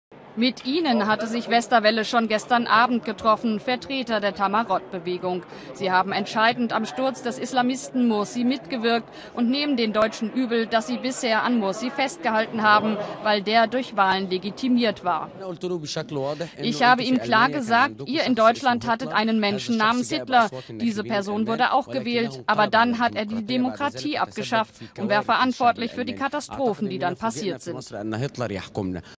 TAMAROD, Ägypten, zu Westerwelle 2. 7. 2013
"Ihr in Deutschland hattet Hitler. Der wurde auch gewählt, aber dann schaffte er die Demokratie ab." (TAMAROD, Ägypten)